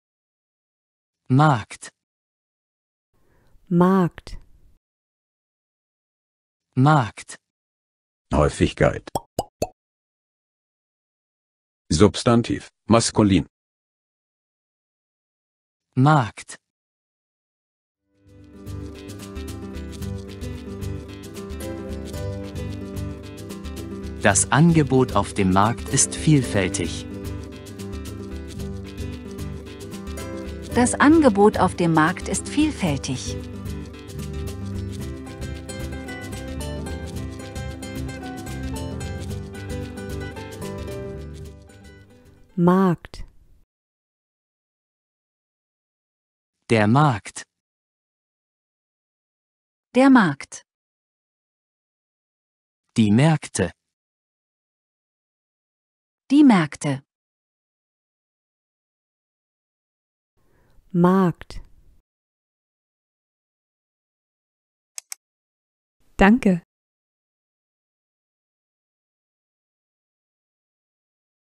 Markt ⭐⭐⭐⭐⭐ Wörterbuch | Deutsch lernen
Mit dem Wörterbuch Deutsch lernen: Markt. Deutsch mit Untertiteln in vielen Sprachen. Mit Dictionary German lernst du die richtige deutsche Aussprache, Singular, Plural und einen Beispielsatz.
0:00 Markt
0:09 Häufigkeit
0:12 Wortart
0:21 Beispiel, Verwendung